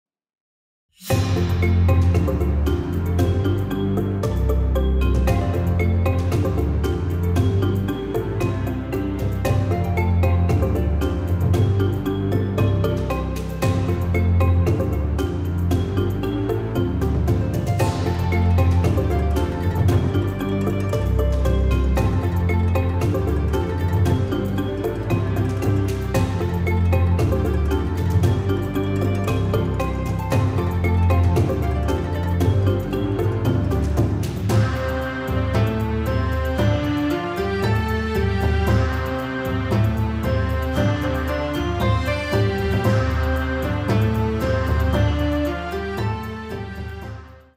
China tem 59 sítios do Patrimônio Mundial com inclusão do Eixo Central de Beijing na Lista da Unesco
O Eixo Central de Beijing foi oficialmente inscrito na Lista do Patrimônio Mundial, na 46ª Conferência do Patrimônio Mundial da Unesco, realizada em Nova Deli, capital da Índia. Até agora, a China tem um total de 59 sítios incluídos.